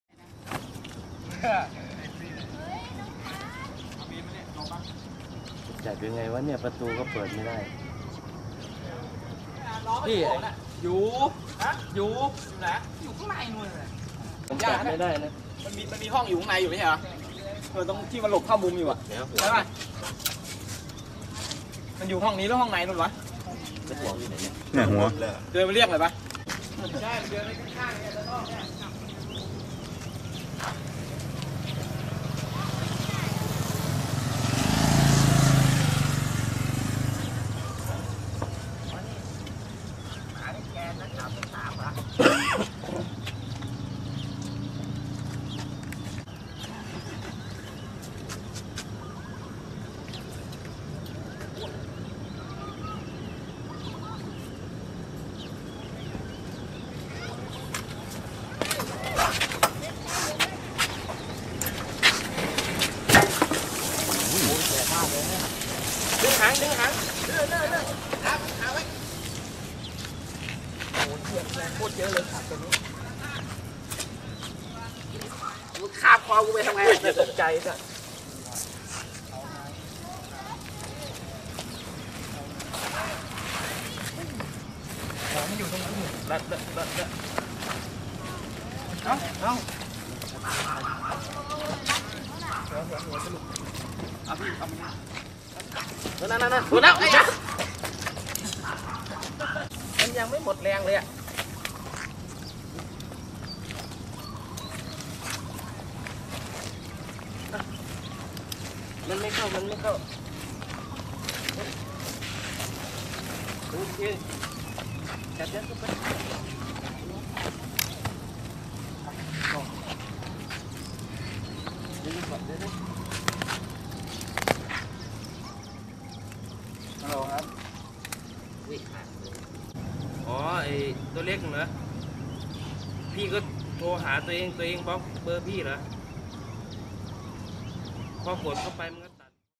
Long Python Sleeping On Factory Fence
Workers were shocked when they arrived for their morning shift and found this giant PYTHON taking a nap on the security fence. The large snake had slithered onto the gate and curled around the top of the concrete corner post in Chonburi, Thailand. It was discovered by a security guard arriving for work at 7am yesterday (Thu) morning. Snake catchers were called to rouse the 12ft long serpent from its slumber. The python, estimated to weight between 30kg and 40kg, reacted angrily and wriggled around before being stuffed inside a bag.